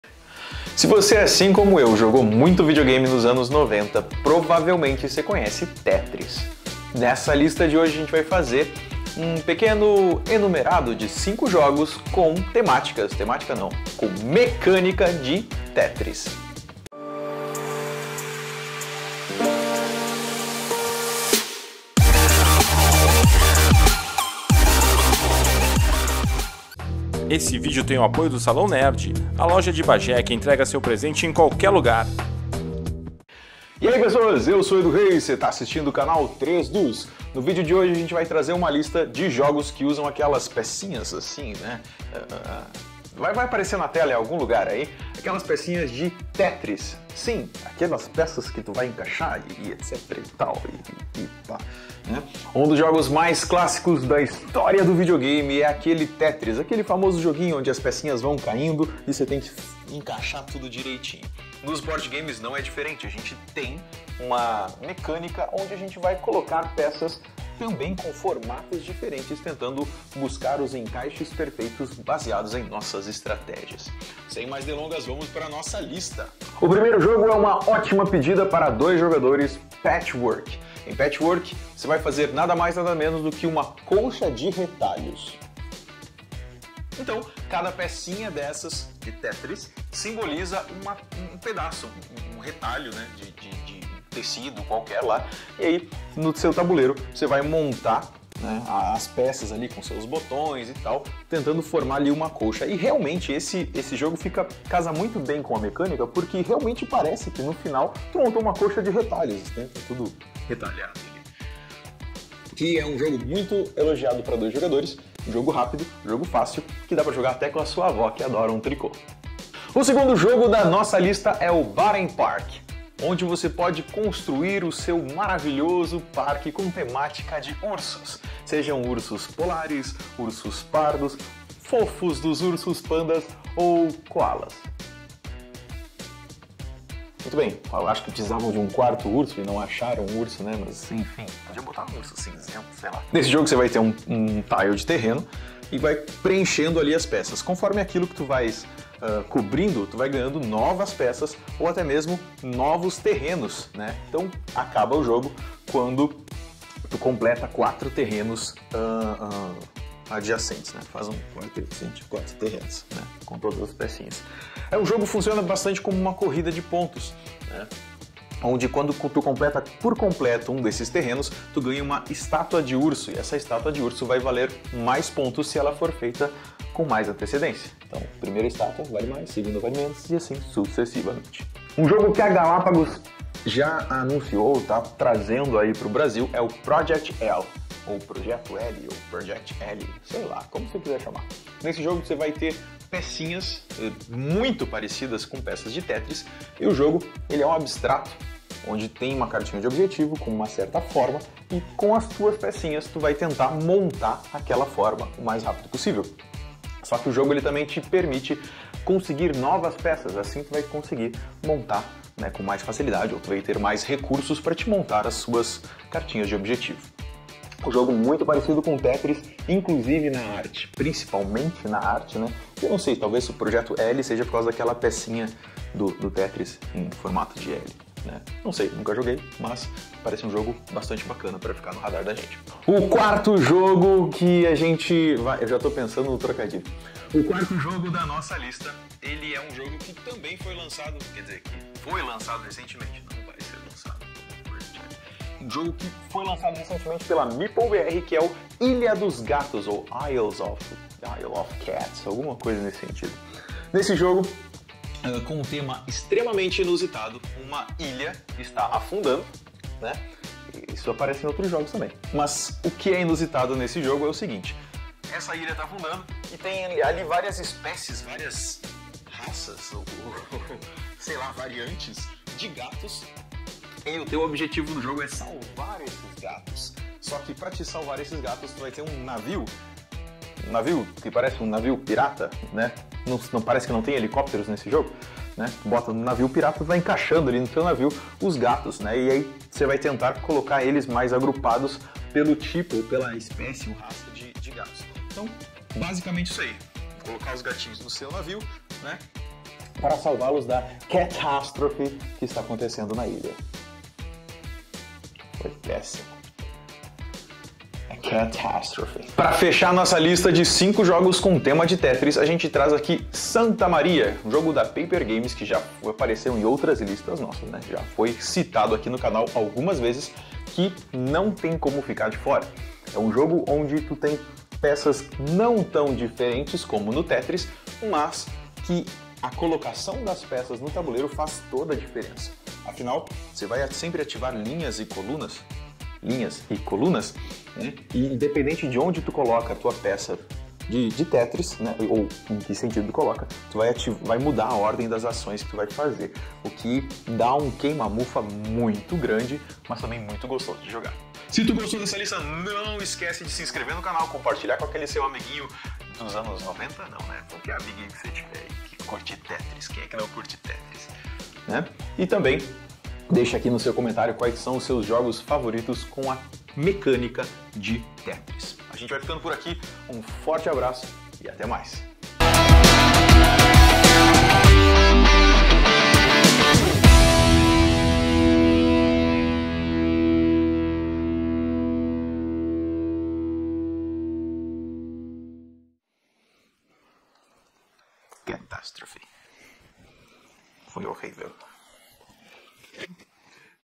5 jogos de tabuleiro estilo TETRIS - Dica dos Edus
No episódio de hoje, Edu Reis encaixa 5 jogos super diferentes em uma lista com uma única coisa em comum: Peças de tetris.